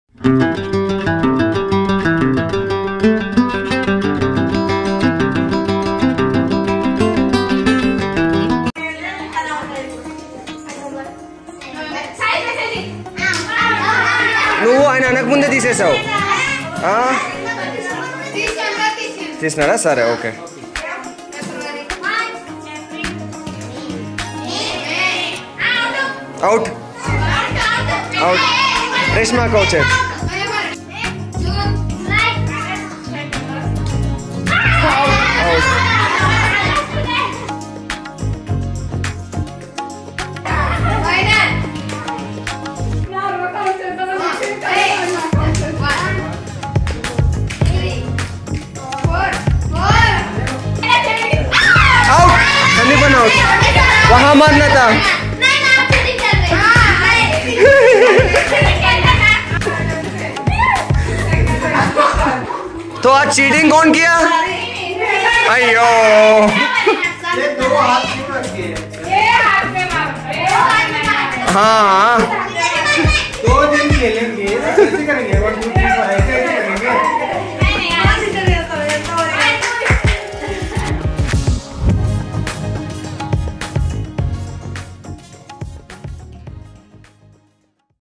One day with children at Pranava Kshetra
When this awesome species take over your day, you can do nothing but surrender... these beautiful souls can teach you things and make you question your existence and fundamentals. It's an honour to serve the children of Mangalgi this way